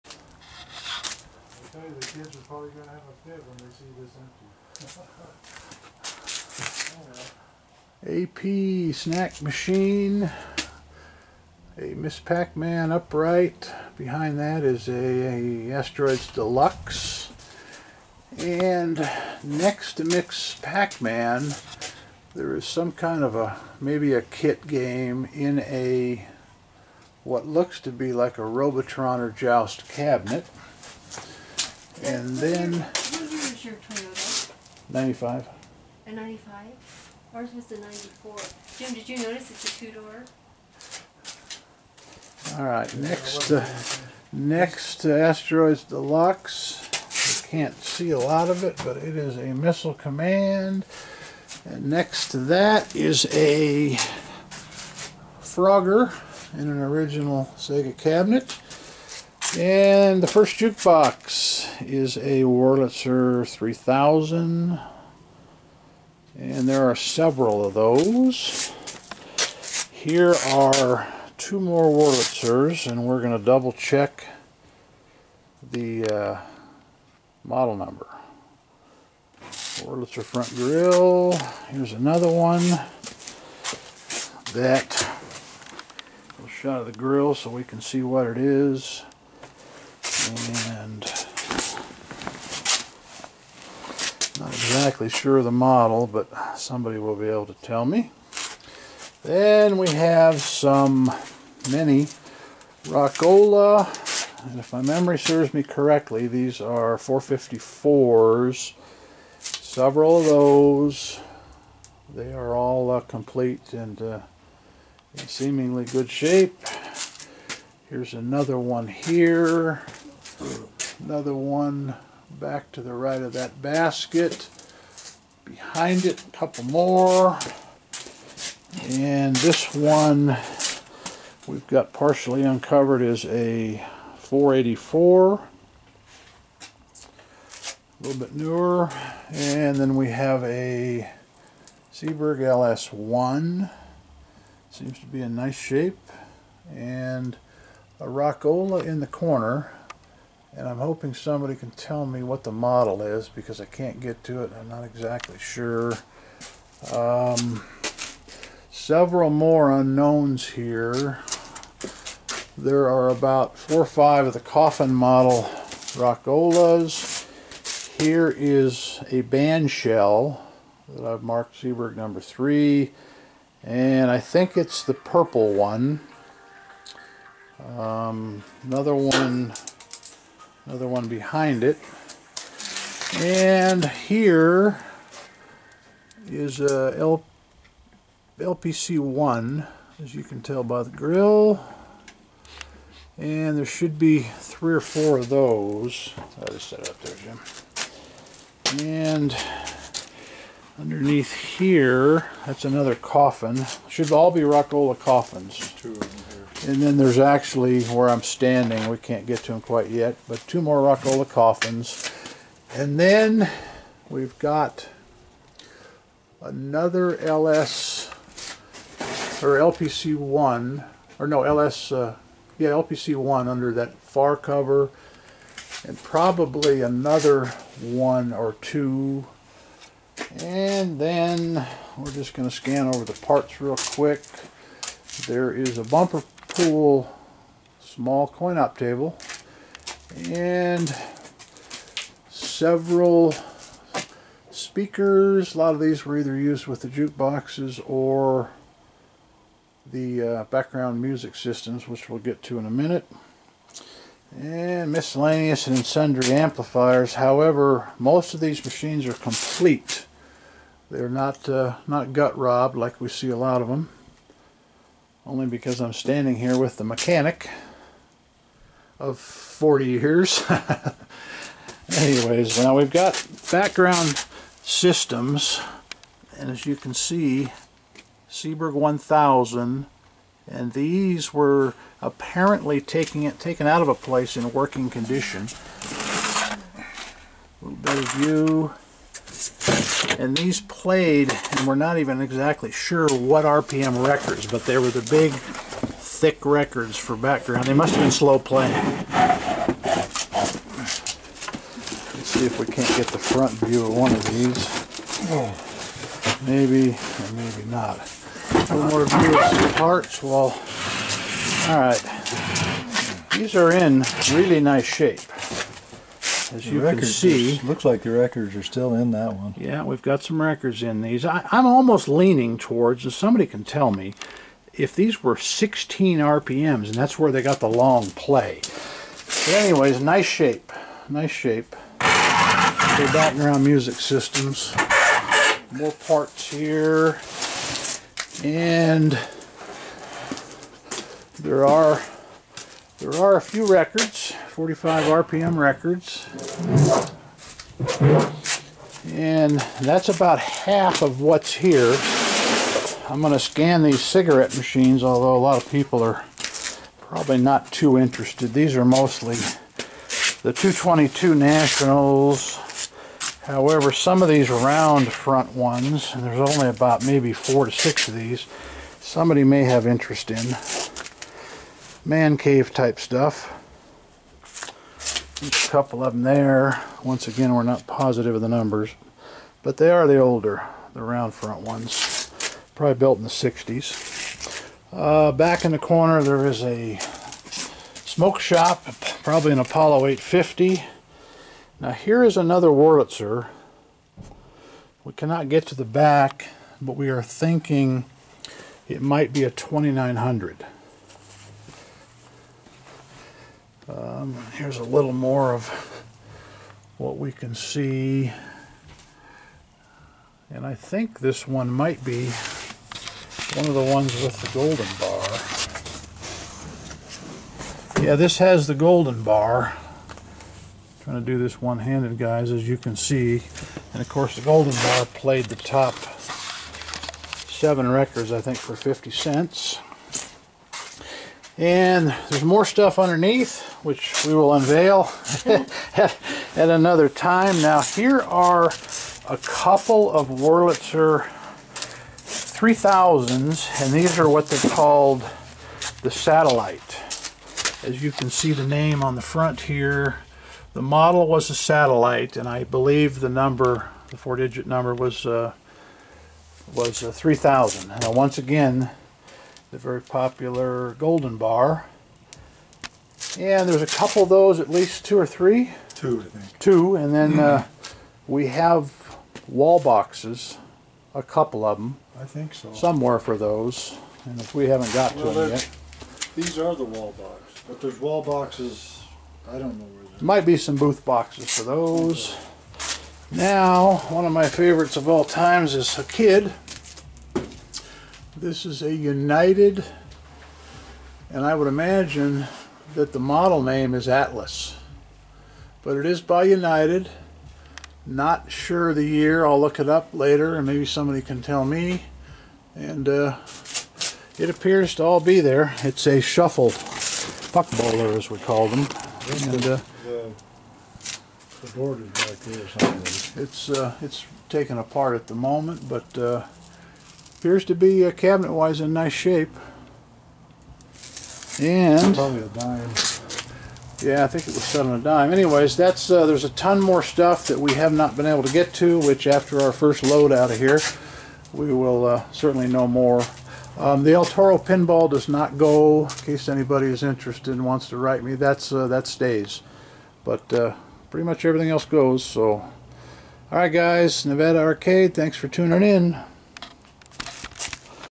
Barn Antique Amusements Jukeboxes, Wurlitzer, Seeburg, Rock-Ola +++
This may be the last barn find of this quality. The amusement operator retired in the late 80s and removed all this equipment from location. 99% of these machines are complete and in the same shape they came off location 25 to 35 years ago. The 26 jukeboxes all appear to still have the 45 rpm records in them. I am told there are a few Wurlitzer booth boxes hidden somewhere in the load, plus speakers. There is also 5 video games, Frogger, Ms Pac Man, Asteroids Deluxe, Missile Command and 1 unknown, ( might be a kit game in a Stargate cabinet ) There are also several classic National Cigarette machines. Everything here is for sale. The items are 90 miles North of Las Vegas ( Beatty NV ) We have a 4 hour window on Nov 27th in the morning to remove everything. Please enjoy, I have bought out well over 100 amusement operators in the last 40 years, but none were as nice and organized as this one, might be the last.

Operate your own channel? The best link you will click this month.